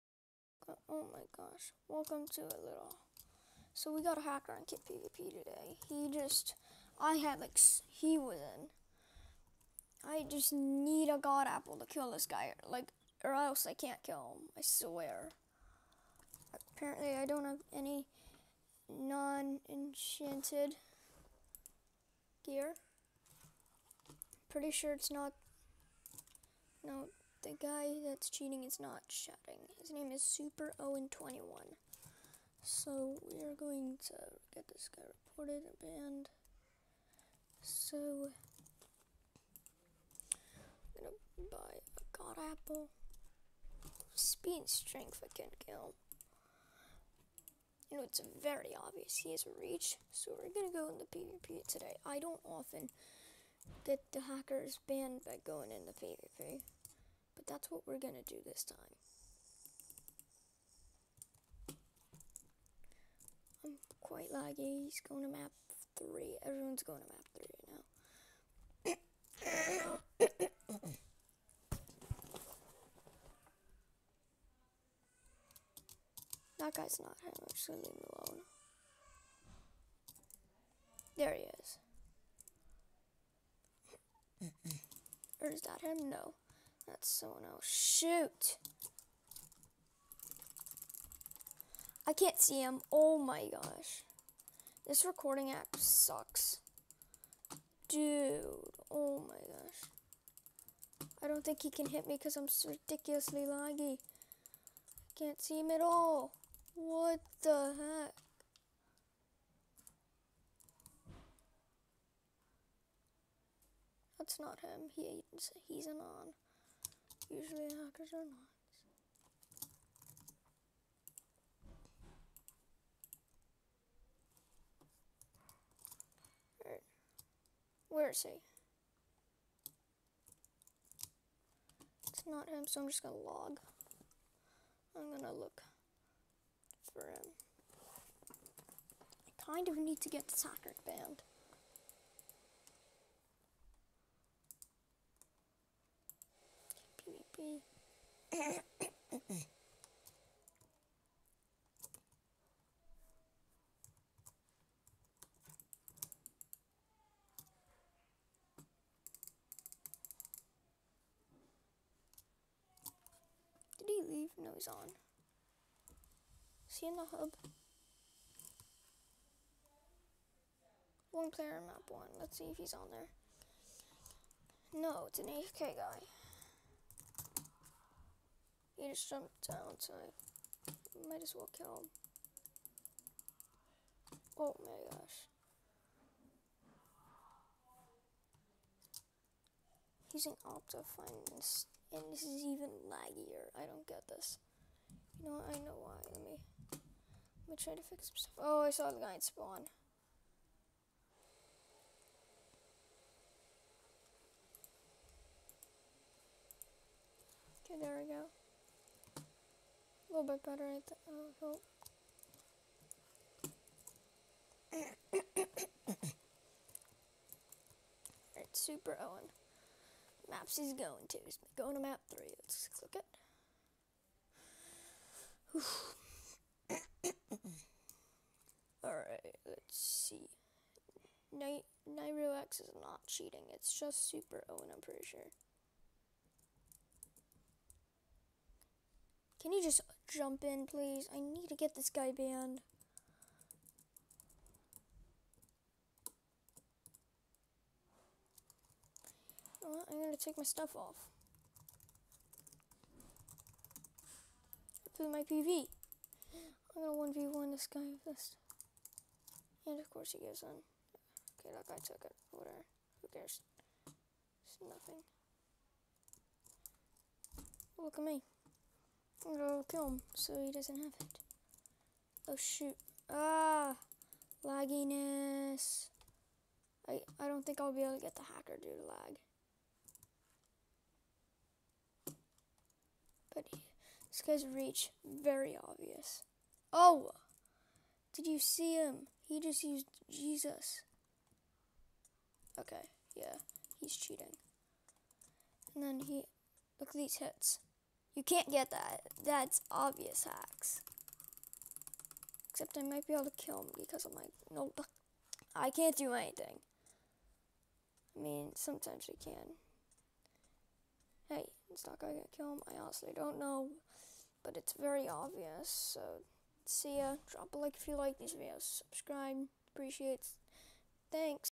Hacker- superowen21
superowen21 says i have reach and can kill anyone!
ilisto says i have bhop and i can totally kill anyone!
wonkagamer383 says hes a god and will kill, and end them all.
hopefully not click bait.